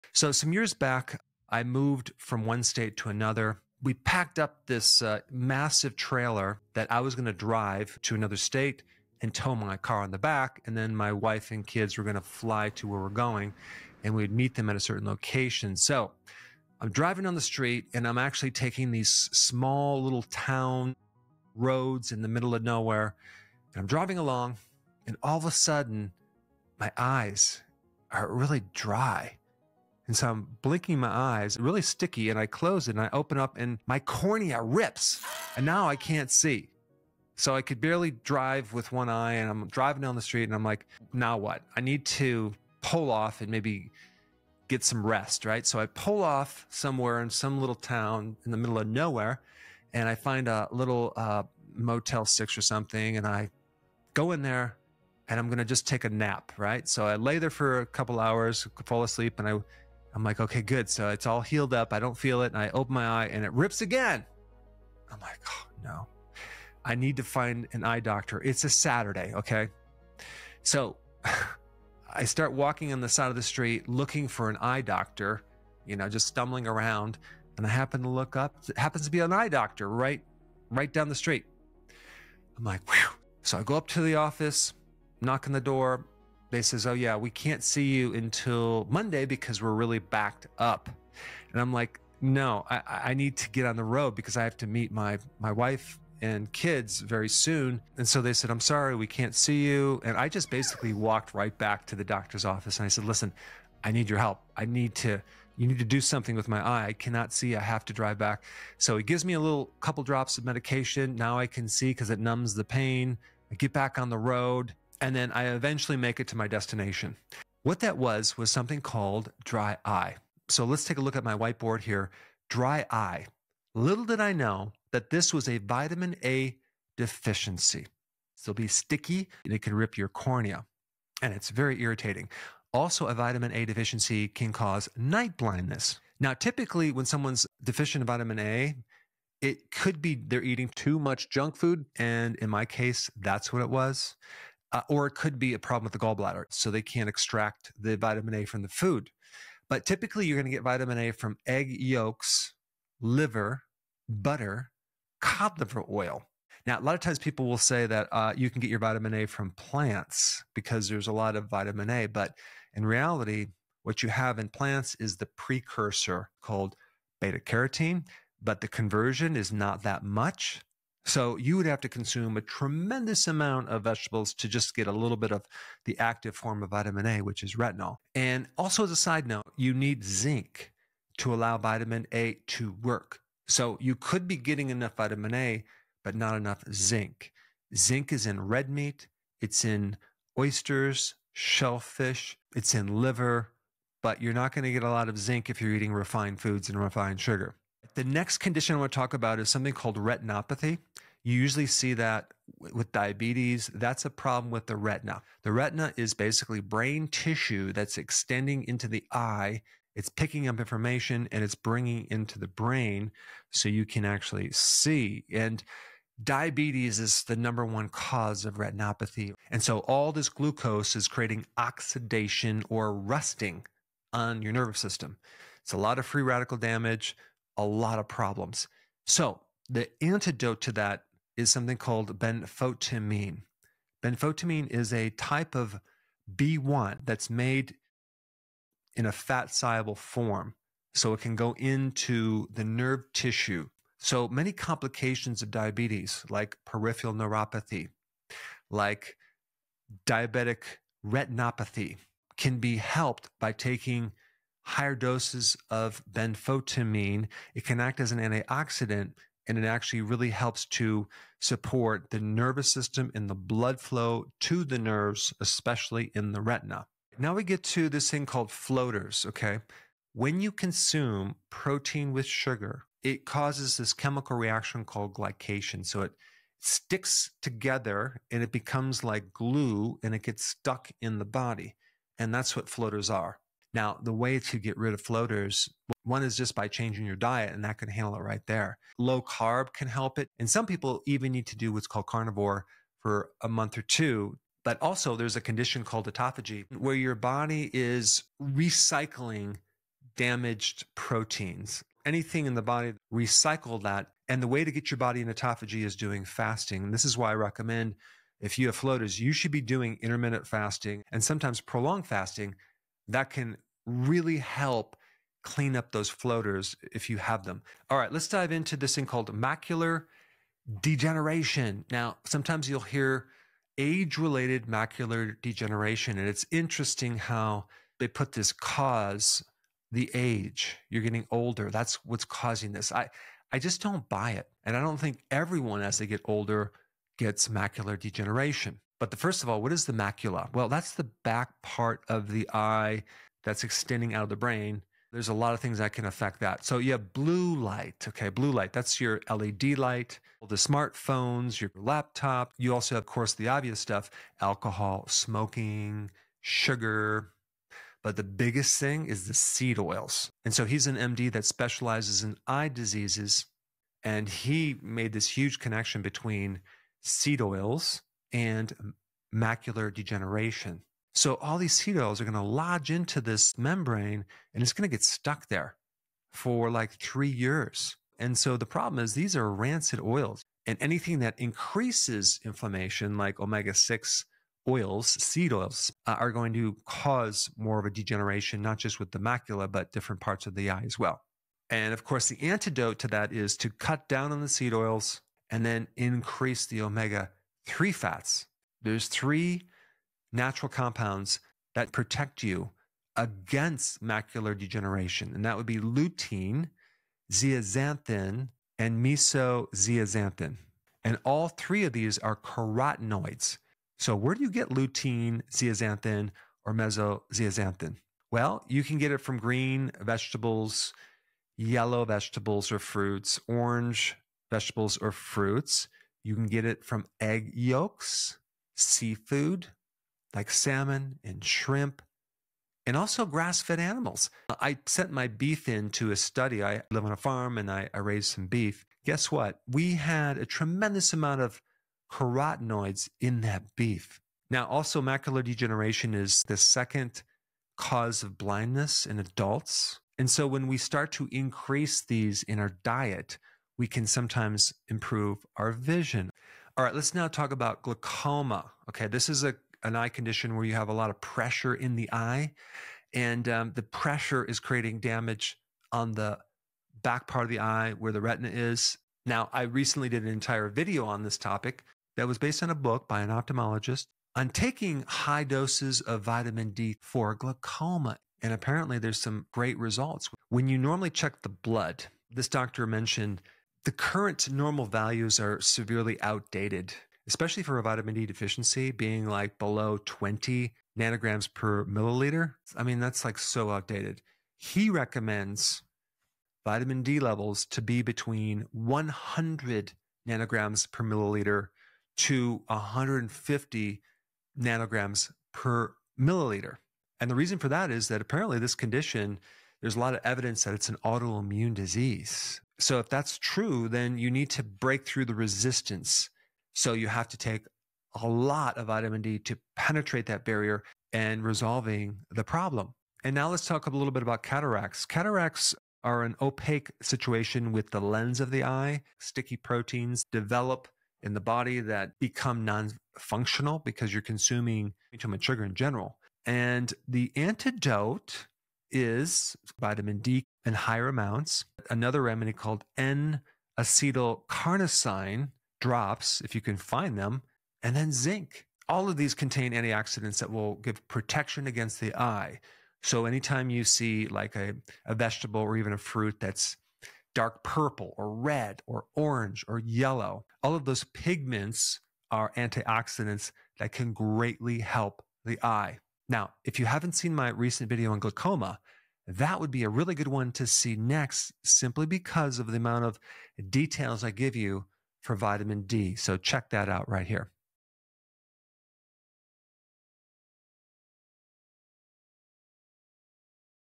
What Causes Eye Floaters and Dry Eyes?
Get rid of dry eyes, floaters, and other problematic issues with the eyes by increasing your intake of these essential nutrients.

0:00 Introduction: Dry eyes symptoms
1:57 What causes dry eyes?
3:17 Retinopathy
4:27 Floaters
5:18 Macular degeneration
7:36 Glaucoma
8:42 Cataracts

In this video, we’ll discuss some common eye problems and the nutrients that can help. Dry eyes and night blindness are often caused by vitamin A deficiency.

Vitamin A deficiency can be caused by a diet high in junk food or an issue with the gallbladder.

Egg yolks, liver, butter, and cod liver oil are the best sources of vitamin A. Plants are not a good source of vitamin A because they only contain the precursor, beta-carotene.

Zinc is essential for vitamin A to work properly in the body. Red meat, oysters, shellfish, and liver are the best sources of zinc.

Retinopathy is often caused by diabetes. Excessive amounts of glucose can create oxidation in the nervous system. Benfotiamine, a fat-soluble form of vitamin B1, can penetrate nerve tissue.

Consuming protein with sugar causes a chemical reaction called glycation, which can cause floaters in the eye. A low-carb or carnivore diet may help. Intermittent and prolonged fasting allow your body to recycle damaged proteins through autophagy and may help with floaters.

Macular degeneration is often said to be age-related. However, blue light, alcohol, smoking, sugar, and seed oils may contribute to it. To improve eye health, try lowering your seed oil consumption and increasing your intake of omega-3 fats.

Lutein, zeaxanthin, and meso-zeaxanthin are carotenoids that protect your eyes against macular degeneration. You can find these compounds in the following foods:
• Green vegetables
• Orange fruits and vegetables
• Yellow fruits and vegetables
• Egg yolks
• Seafood (Salmon, shrimp)
• Grass-fed meat

Glaucoma is an eye condition in which excessive pressure causes damage to the back of the eye. Research has shown that vitamin D can be an excellent remedy for glaucoma. High amounts of vitamin D can also be beneficial for cataracts.

N-acetyl-carnosine and zinc contain antioxidants that protect the eye and improve cataracts. Dark purple, red, orange, and yellow fruits and vegetables contain antioxidants that help support eye health.

I hope this has increased your awareness about the most important vitamins for the eyes.